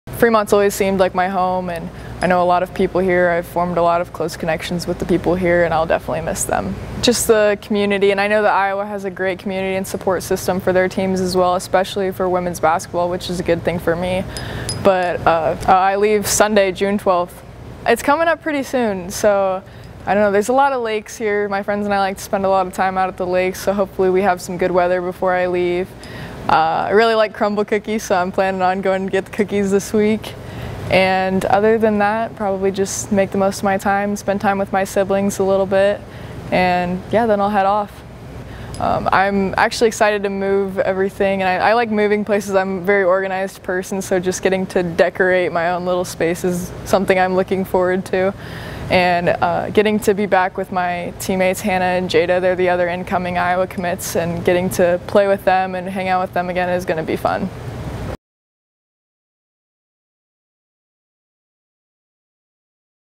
Fremont's Taylor McCabe getting ready to head to Iowa
Fremont's Taylor McCabe preparing to take her career to head to Iowa